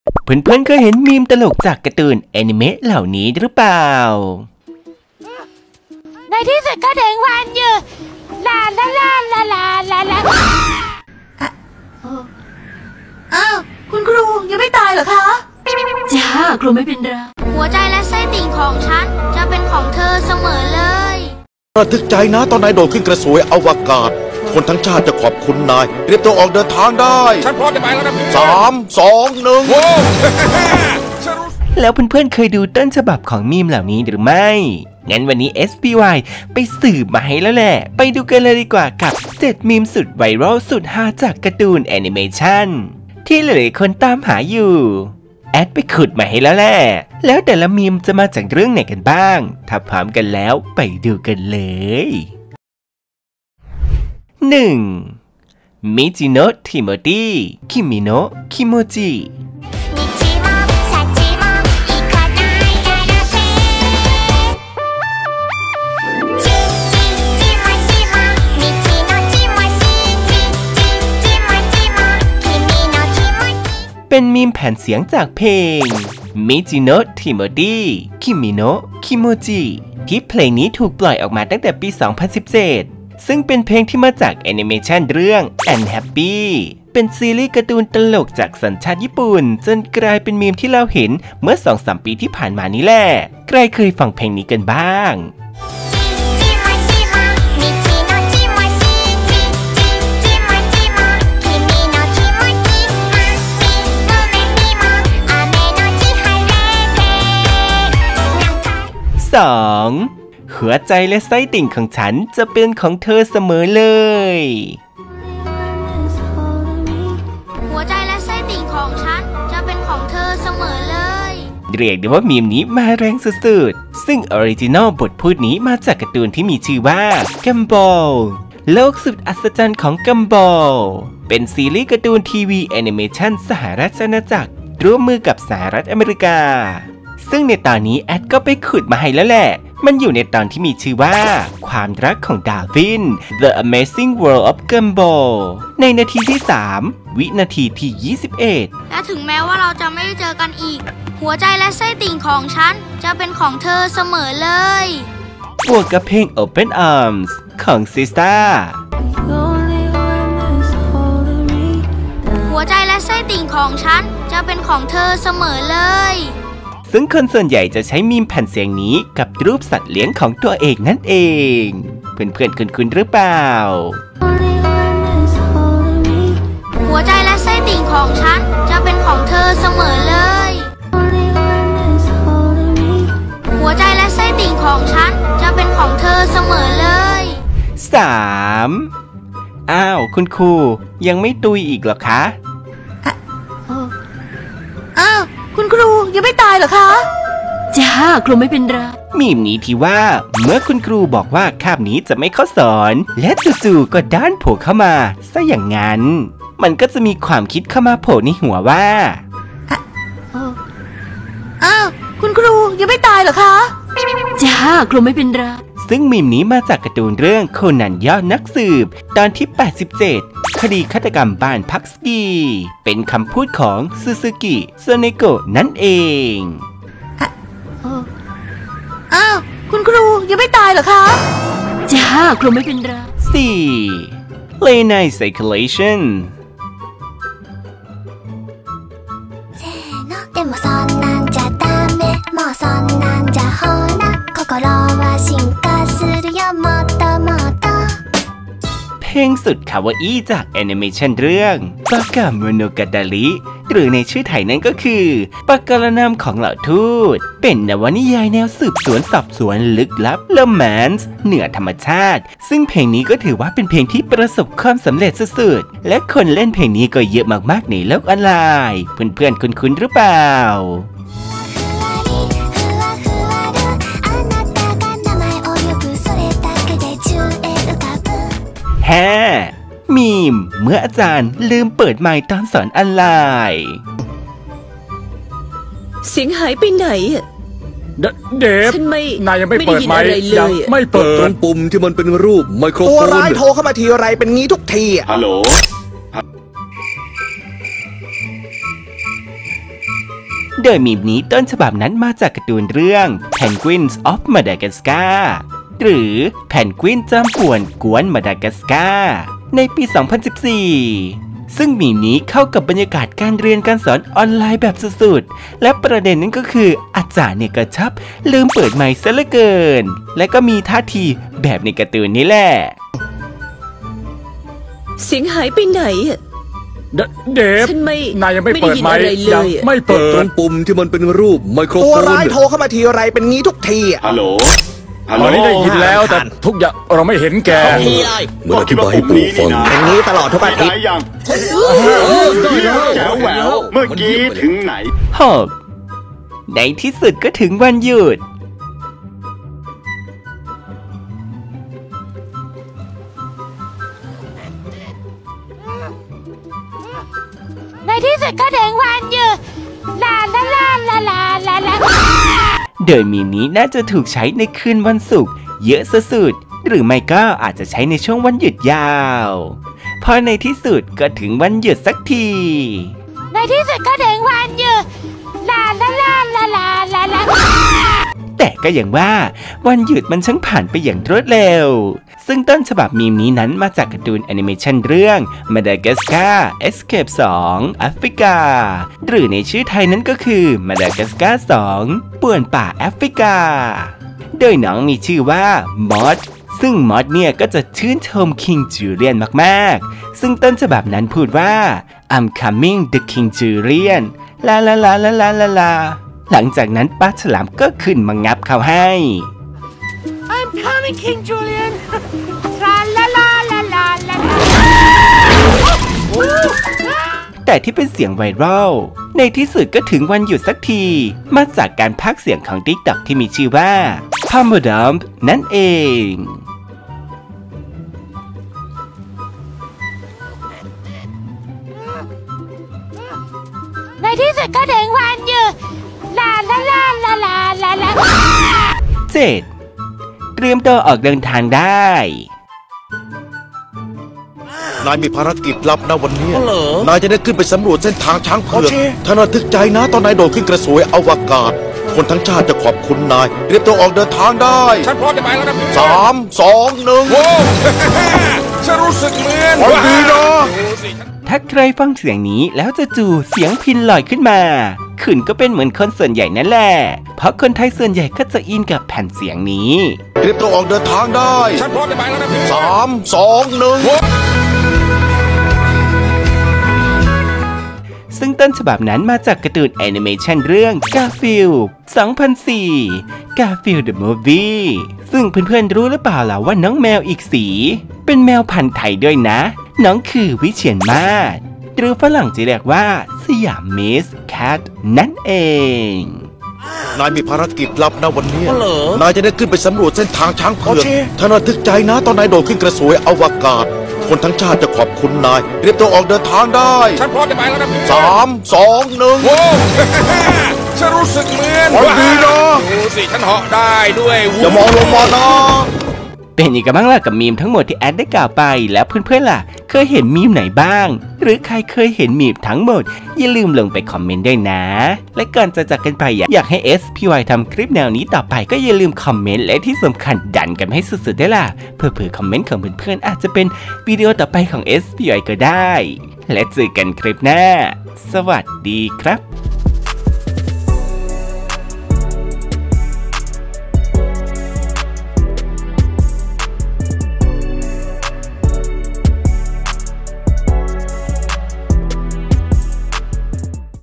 7 ต้นฉบับ “มีมไวรัล” จากการ์ตูน หลายคนตามหา (เจอสักที!!)
เพื่อนๆ
เคยเห็นมีมตลกๆ
จากการ์ตูน อนิเมะเหล่านี้หรือป่าว?